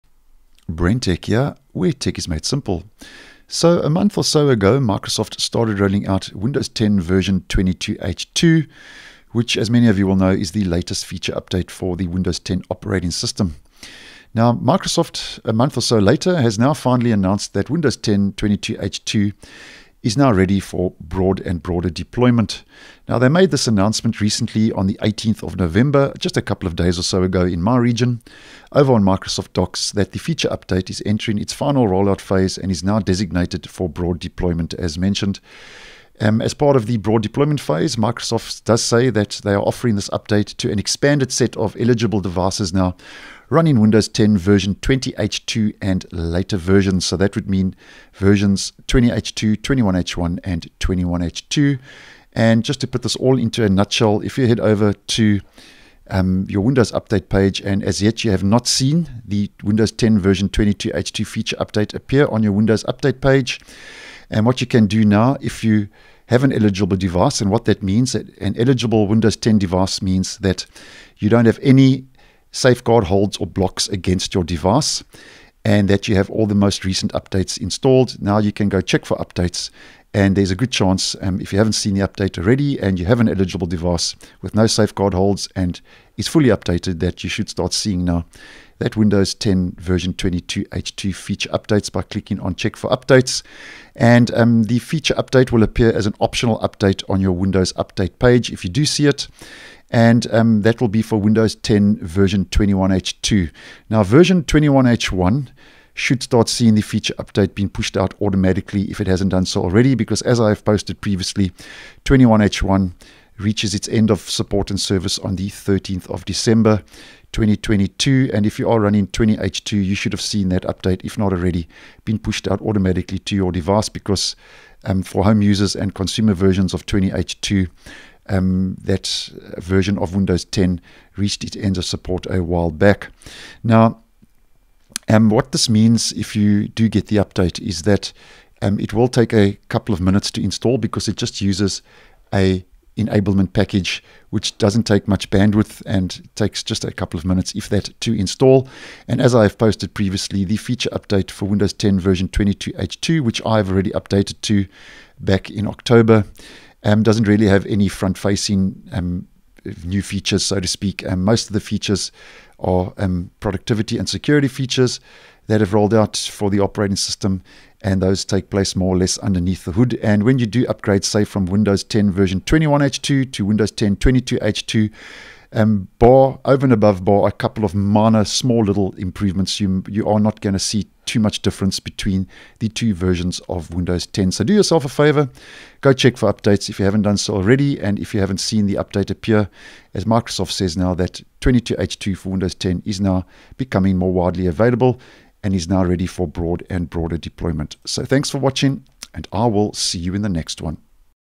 Windows 10 22H2 is now ready for broad deployment
Microsoft has recently announced that the feature update is entering its final rollout phase and is now designated for broad deployment. As part of the broad deployment phase, Microsoft is offering this update to an expanded set of eligible devices running Windows 10, version 20H2 and later versions. So if you have not seen the upgrade yet, go check for updates.